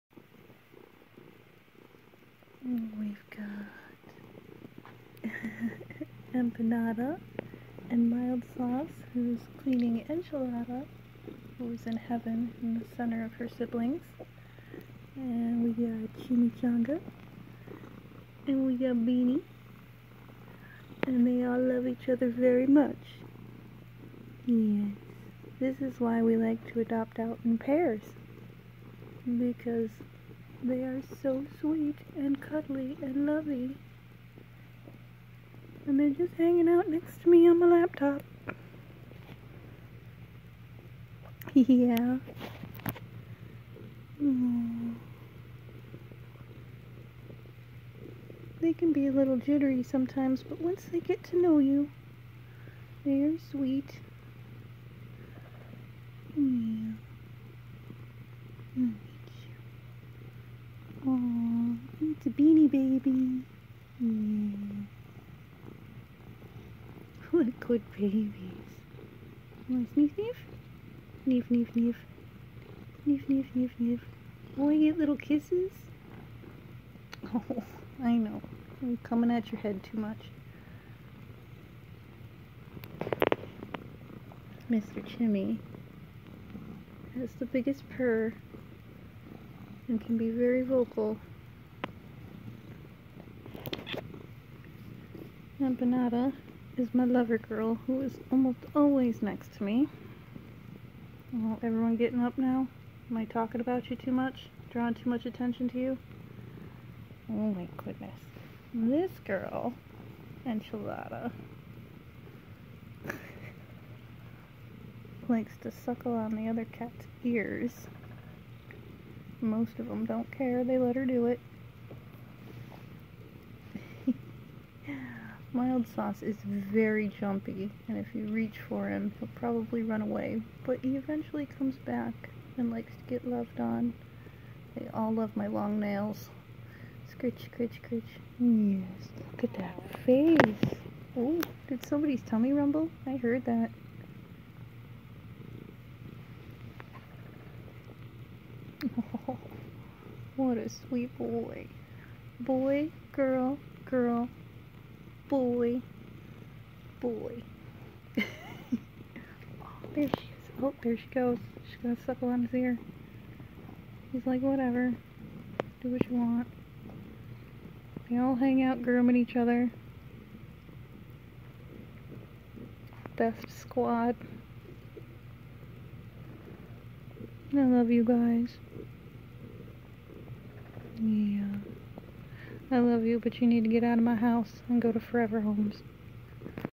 Taco Bell kitten cuddle puddle
all these kittens need forever homes, preferably in pairs as they all love each other so much. they are about 9 months old now. all fixed, dewormed, vaccinated, tested and microchipped!!! adoption fee is $120 for a pair

available through Fur Angels Rescue Shelter in Toledo Ohio